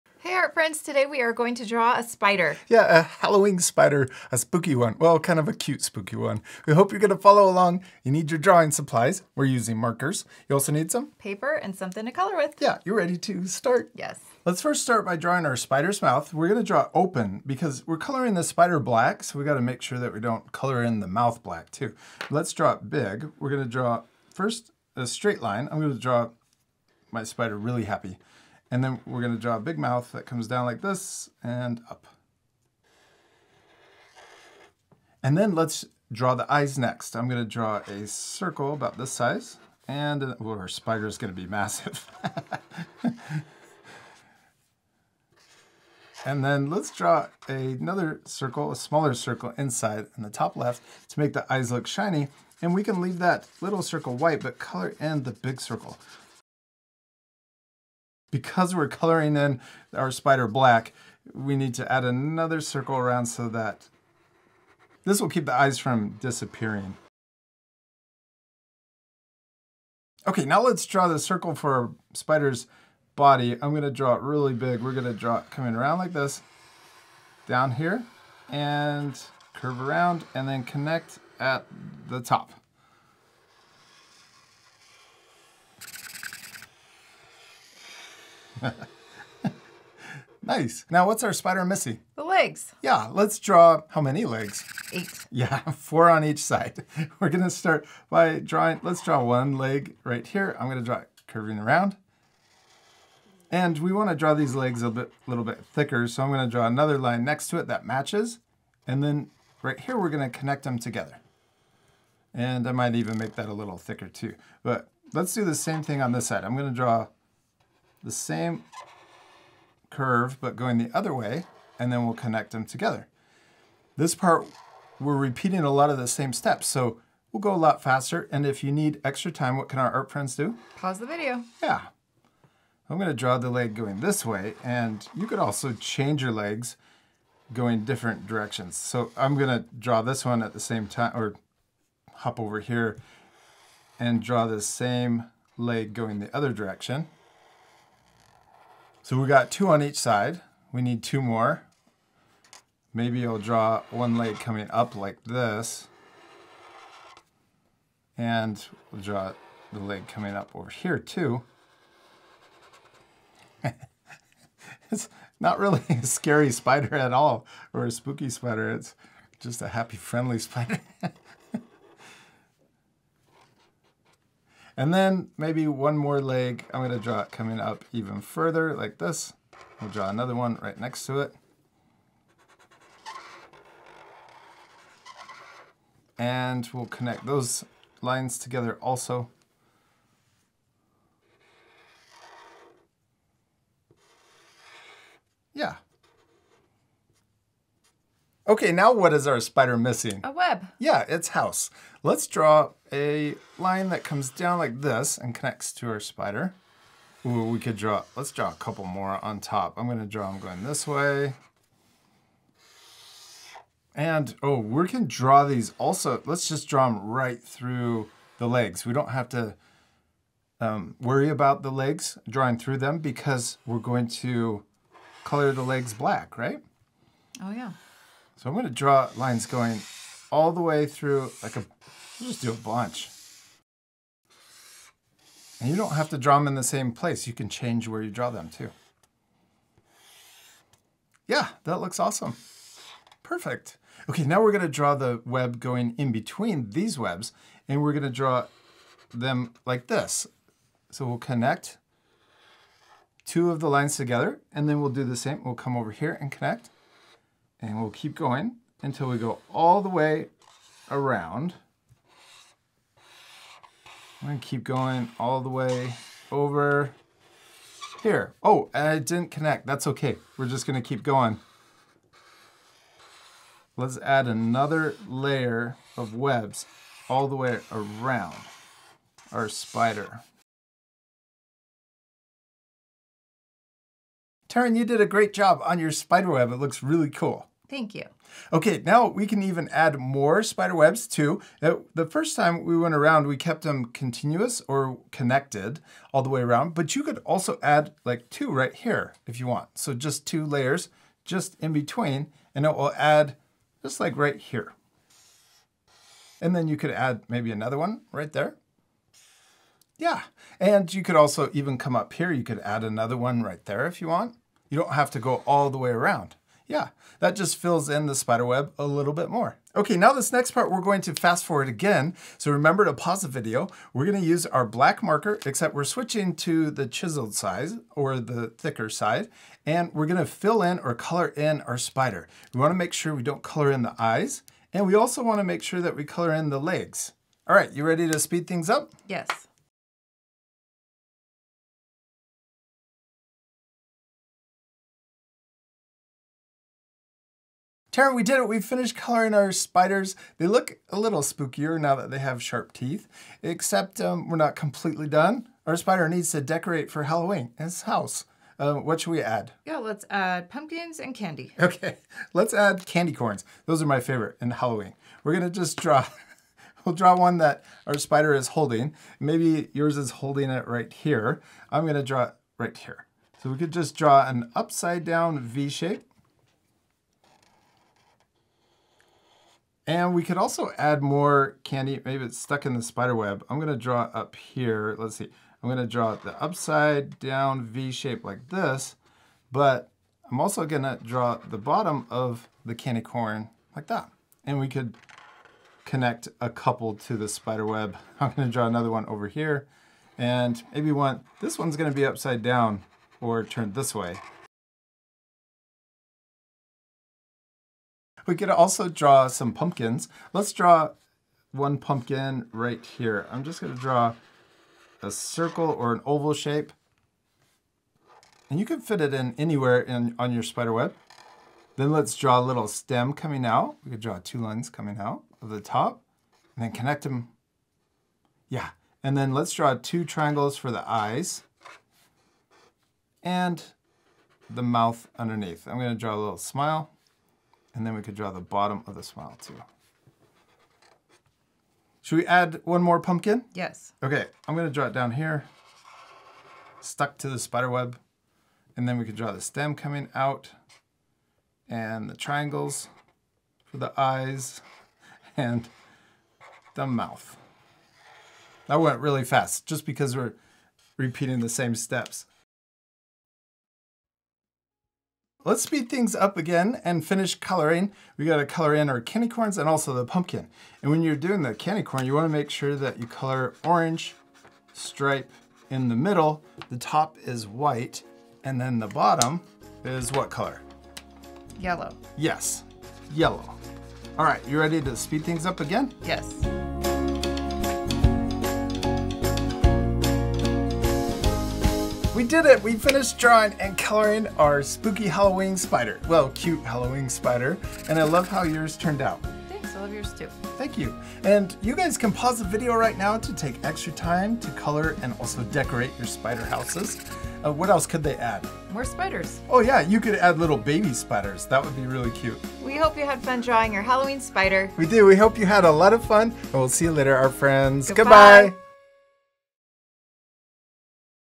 How To Draw A Halloween Spider
Hey, art friends! Today, Teryn and I are getting ready for Halloween by drawing something super fun—a Halloween spider and its web! 🕷️🕸️ This lesson is great for artists of all ages. Feel free to change up your spider web designs to make it your own! Remember to pause the video if you need more time. Let's get drawing! ❤️💛💙

~Black marker (or something to draw with)